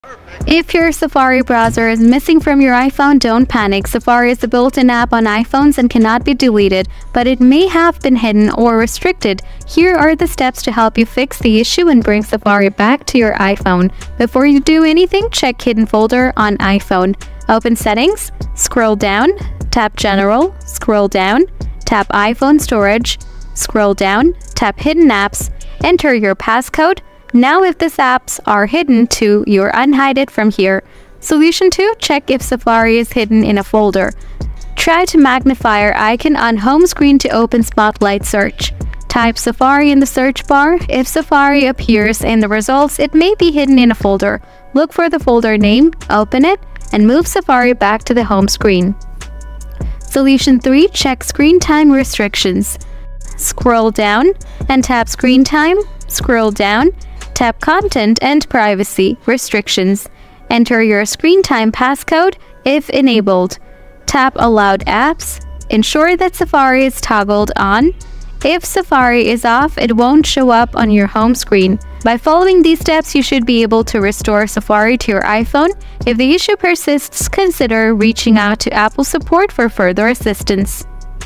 How To Fix Safari Browser Missing On iPhone | Get back Safari App on iPhone Home Screen
Looking to recover the missing Safari App on your iOS 18 device? Look no further! This video will help you restore your Safari back on your iPhone! Well, your Safari hasn't actually disappeared from the iOS home screen. It's most likely there but hidden because of some app settings. Changing those settings usually solves the missing Safari issue on iPhones but if it's a bug or something similar then a full troubleshooting is necessary to get your app back.

I have covered most of the important solutions you can use to get your Safari app back so you don't need to worry about missing any fixes. Just follow my guidelines and you're good to go!

00:01- Video Intro
00:15- Solution 1: Check Hidden Apps
00:32- Solution 2: Check Hidden Folder
00:50- Solution 3: Check Restrictions
01:16- Solution End